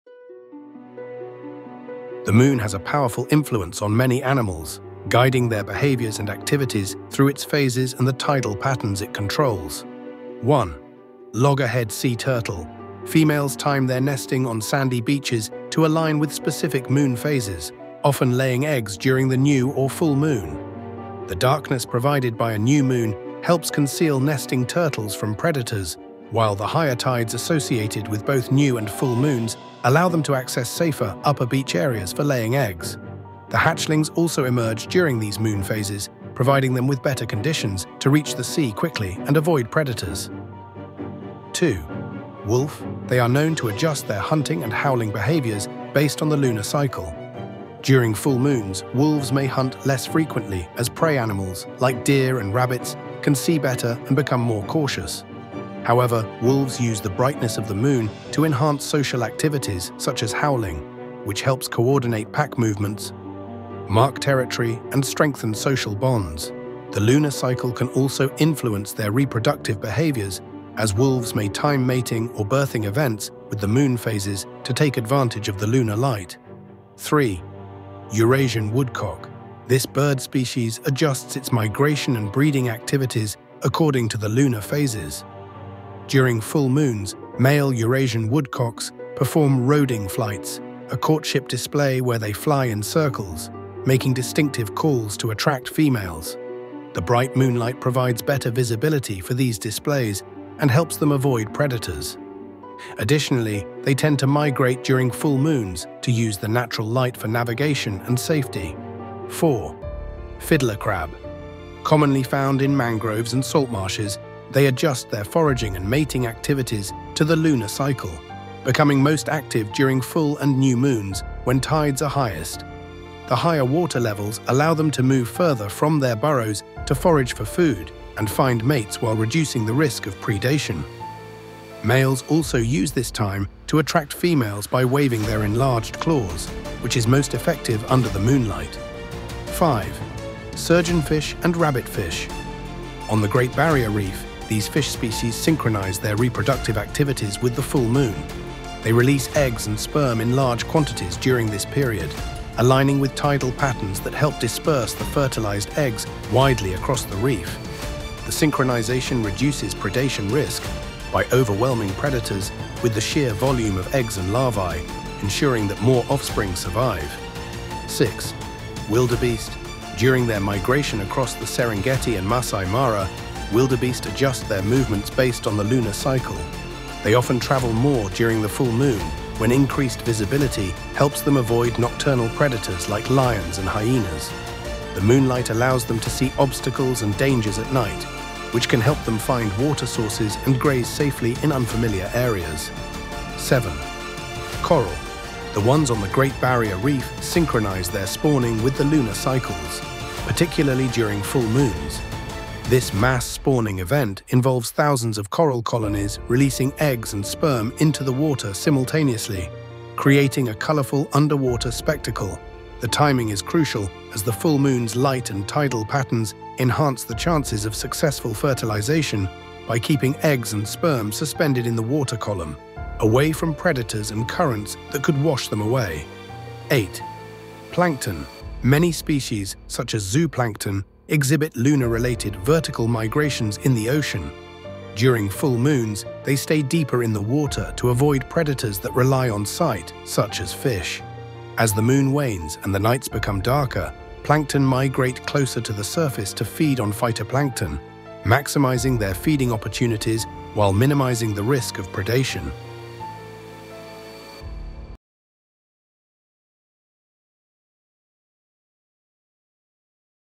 8 Animals That Change Their Behavior Based on the Moon
Did you know that the moon plays a huge role in shaping animal behavior? From sea creatures to land mammals, many animals adjust their activities based on lunar cycles.

In this video, we'll explore 8 amazing animals whose behaviors are directly influenced by the phases of the moon. Watch how these creatures use moonlight to time nesting, hunting, mating, and more! Don't miss this fascinating journey into the rhythms of nature!

✨ Featured Animals:

🐢 Loggerhead Sea Turtle: Females time their nesting and hatchling emergence with the new and full moon phases to avoid predators and access safer nesting spots.

🐺 Wolf: Wolves adjust hunting and howling based on moonlight, using brighter nights for social bonding and territory marking.

🦅 Eurasian Woodcock: These birds perform courtship flights and migrate during full moons to avoid predators and improve visibility.

🦀 Fiddler Crab: These crabs time their foraging and mating to the full and new moons, taking advantage of higher tides.

🐟 Surgeonfish & Rabbitfish: On the Great Barrier Reef, these fish synchronize their reproduction with the full moon to increase offspring survival.

🐃 Wildebeest: During migration, wildebeest use moonlight to navigate and avoid predators during the full moon.

🪸 Coral: Great Barrier Reef corals release eggs and sperm in mass during full moons, timing with tidal patterns for successful reproduction.

🌊 Plankton: Plankton adjust their vertical migration based on the moon's brightness, affecting the entire food chain.

Captions:
0:00 - Intro
0:11 - Loggerhead Turtle
0:42 - Wolf
1:18 - Eurasian Woodcock
1:51 - Fiddler Crab
2:21 - Surgeonfish & Rabbitfish
2:49 - Wildebeest
3:15 - Coral
3:47 - Plankton

Discover how these incredible animals adapt their lives to the moon’s cycle!